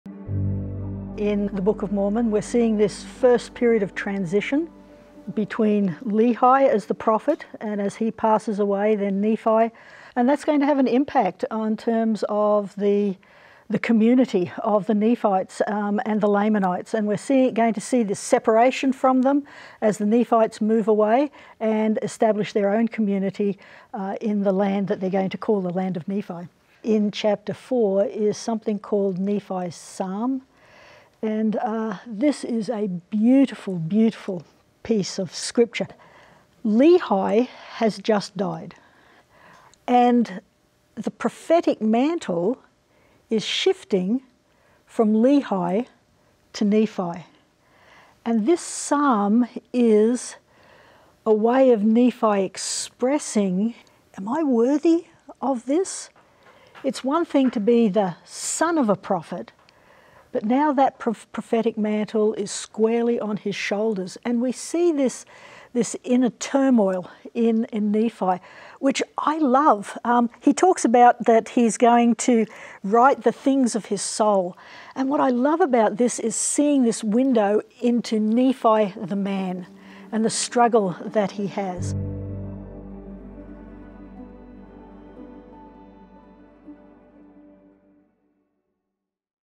2 Nephi 3-5 - Context
Gaye Strathearn presents the background of Nephi’s psalm and his receiving the prophetic mantle.